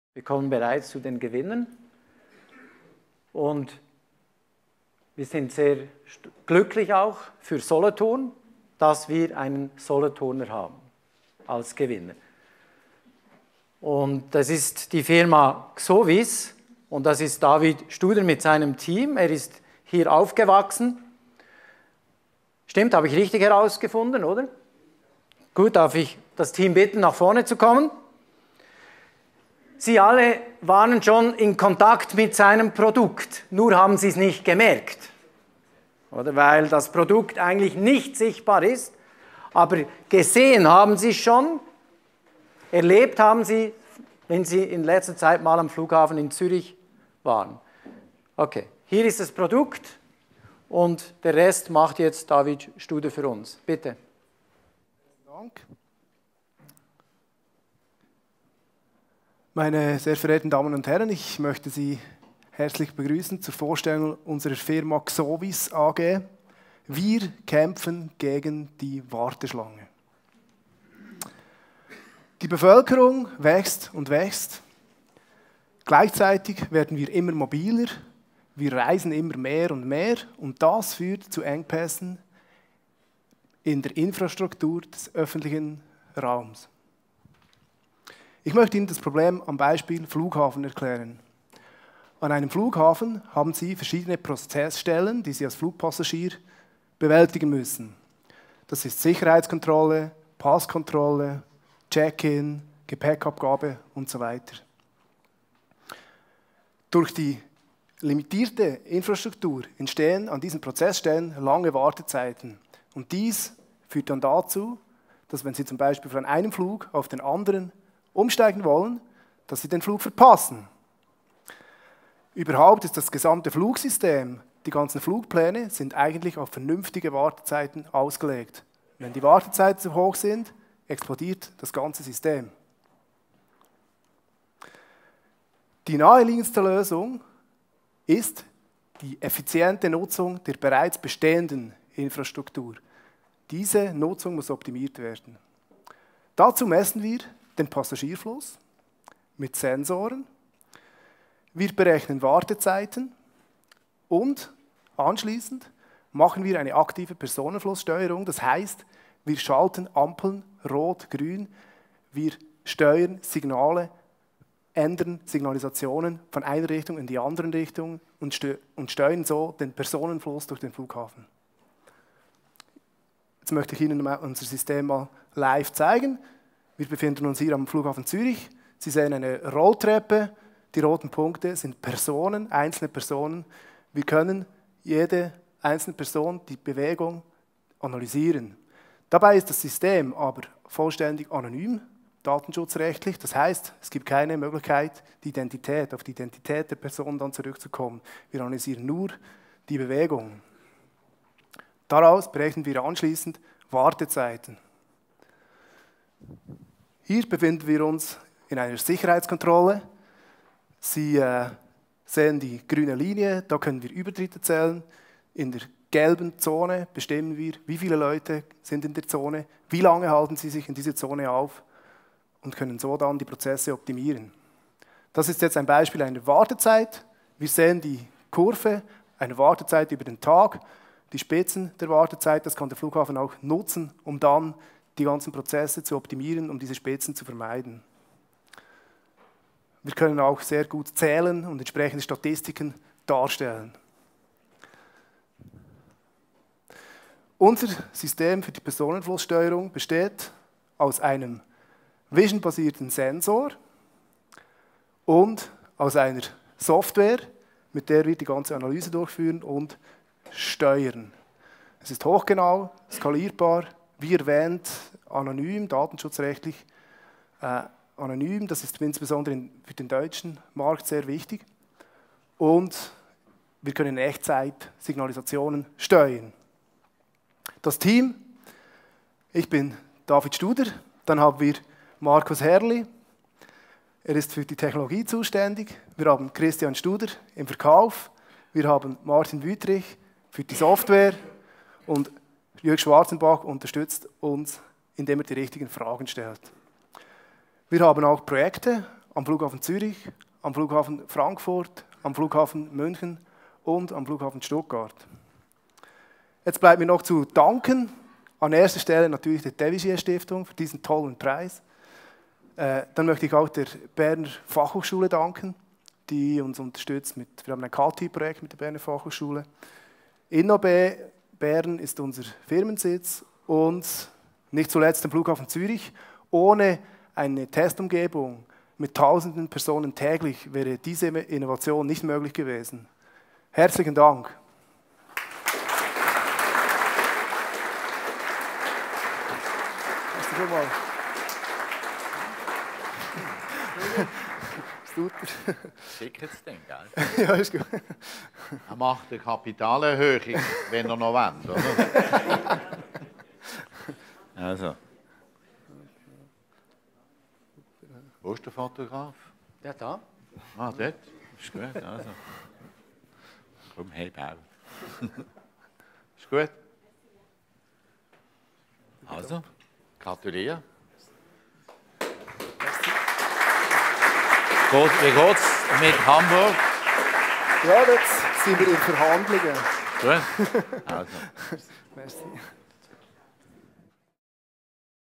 XOVIS - W. A. de Vigier Preisträger 2012
Xovis hat einen Personenzählsensor entwickelt, der betriebliche Abläufe optimieren hilft: Er analysiert Bewegungen und rechnet Wartezeiten aus.